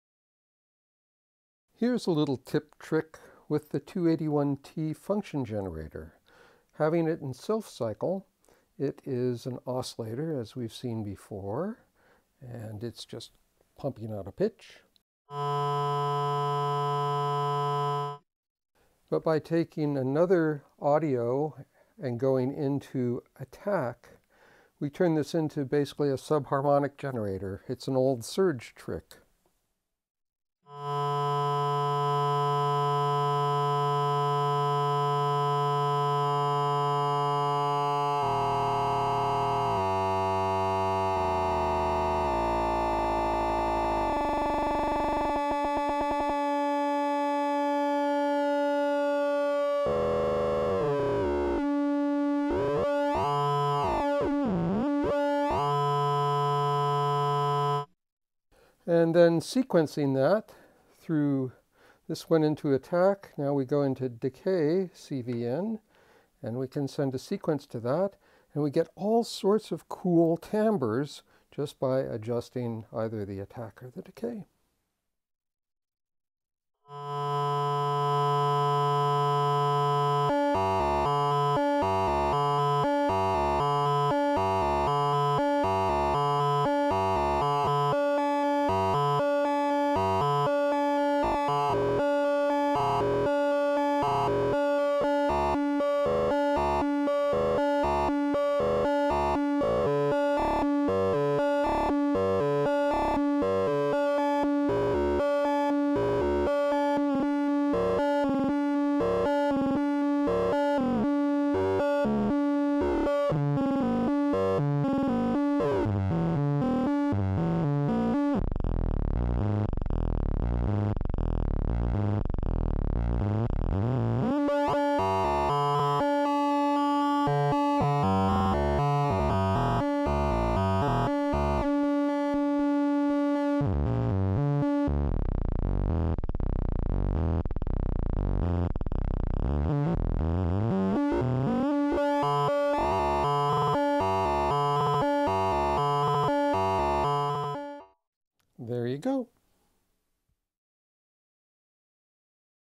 Tiptop 281t Subharmonics
Exploring the subharmonic function of the Tiptop Buchla 281t.